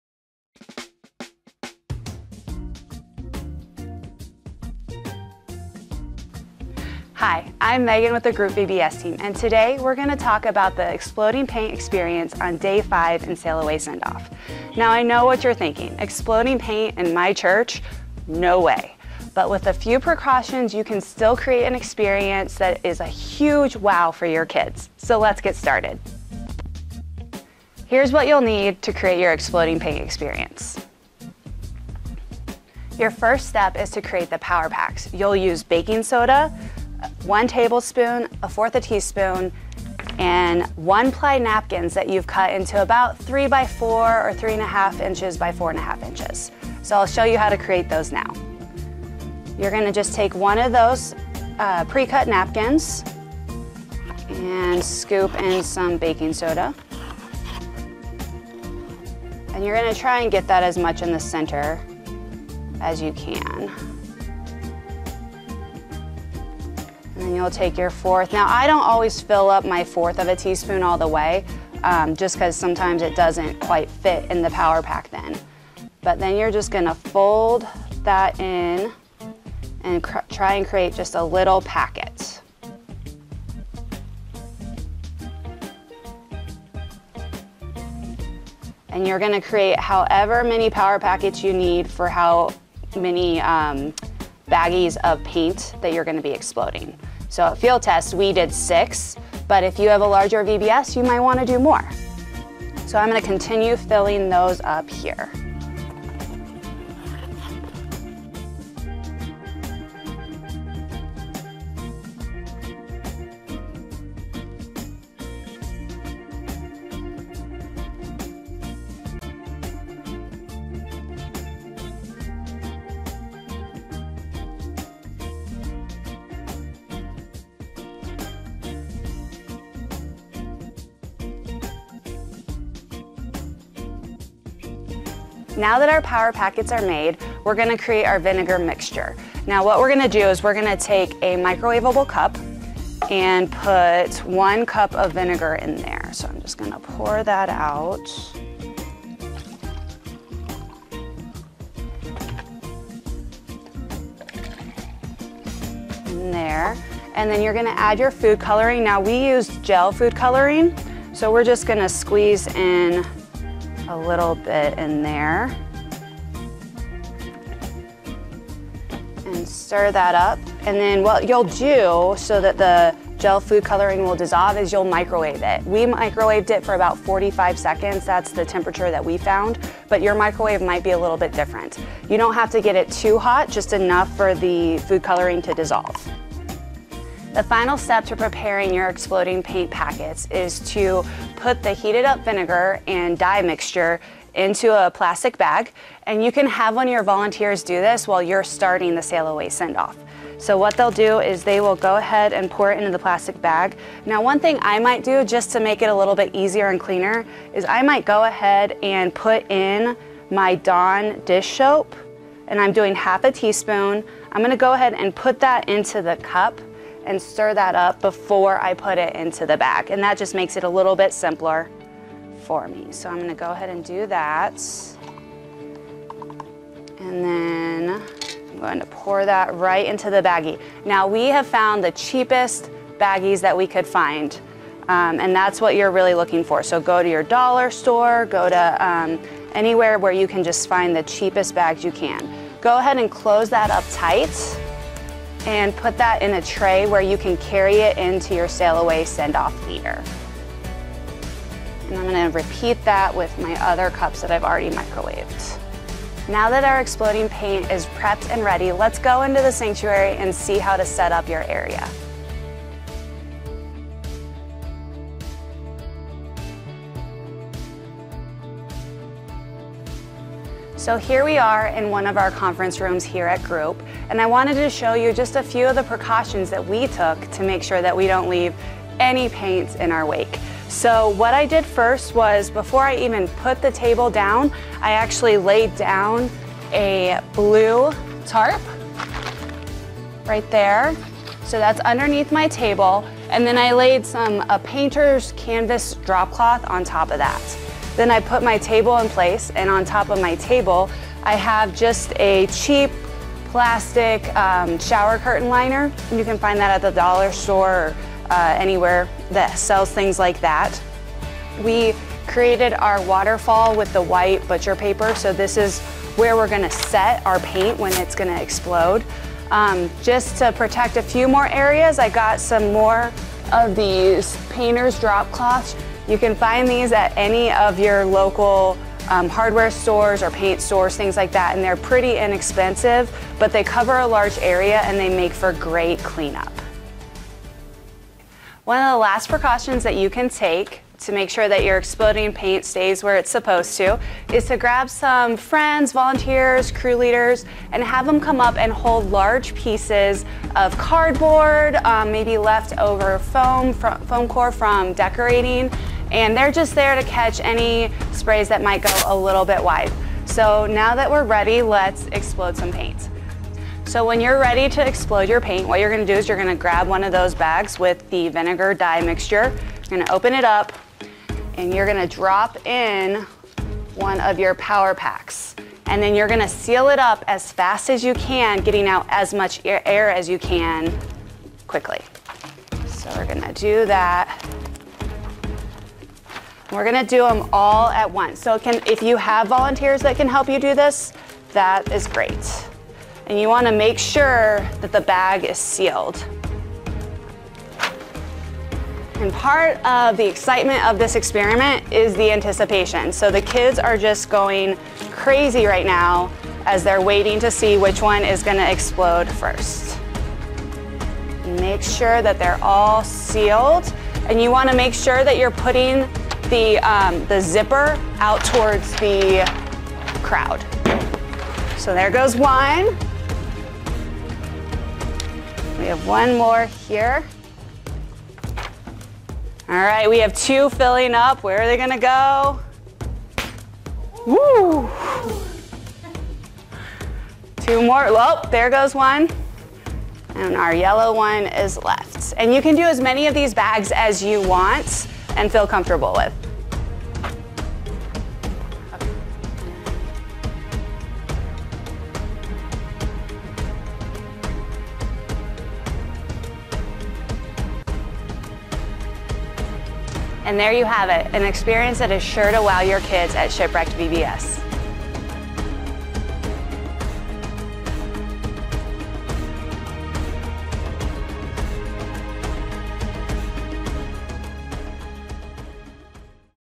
Shipwrecked | Exploding paint
On Day 5 of Sail Away Sendoff, you'll have kids on the edge of their seats with the Exploding Paint experiment. Check out tips and tricks on how to set up the experiment!